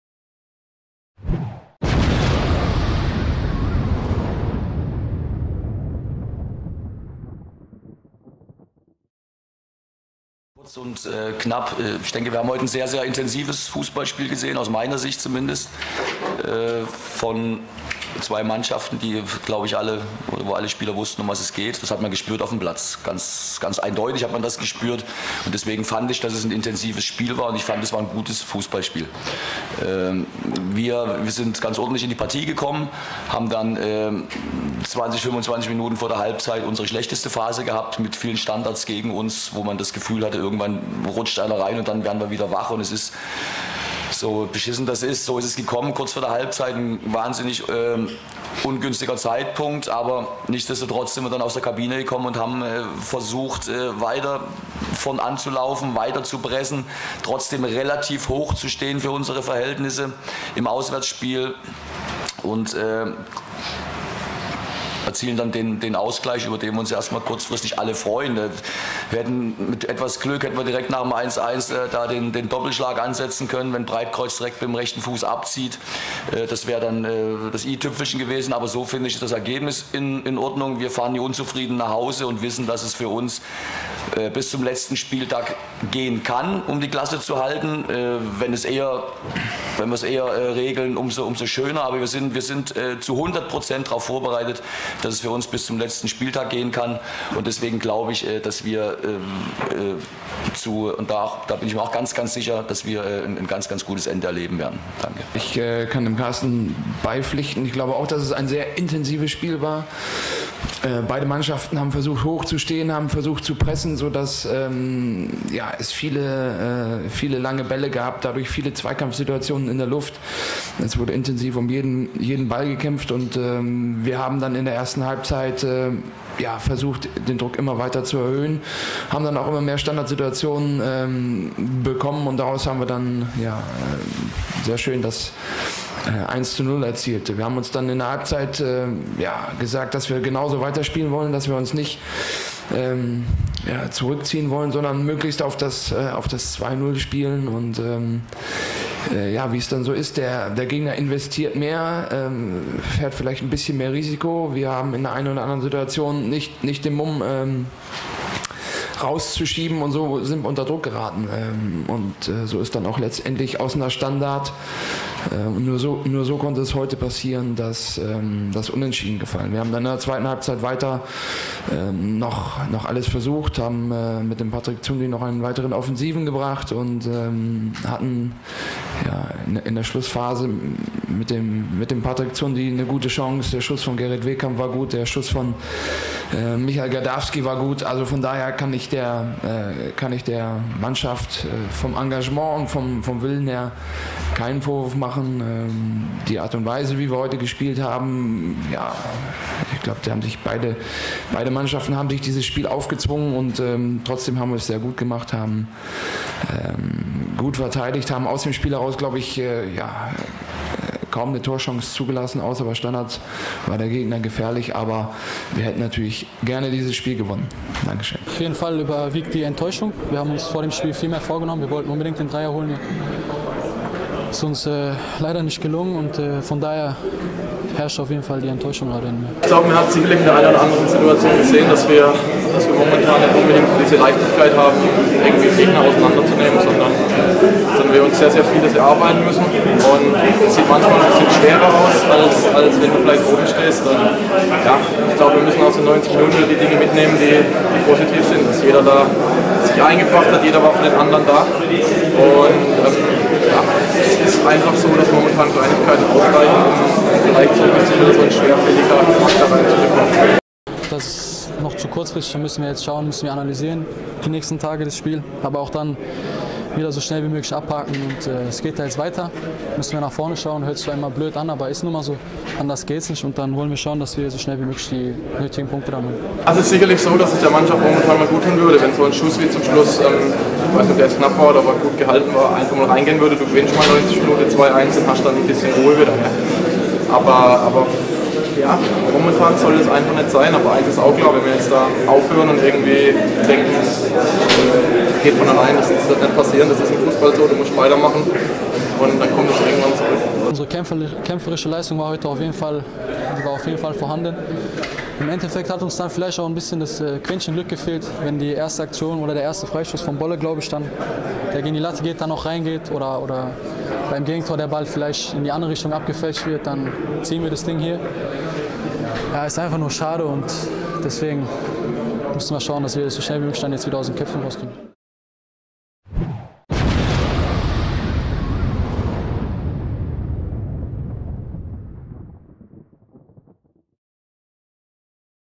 Stimmen Holstein Kiel 140315
Die Pressekonferenz nach dem Spiel mit Karsten Neitzel und Karsten Baumann sowie den Statements von Marcel Lenz und Christian Eichner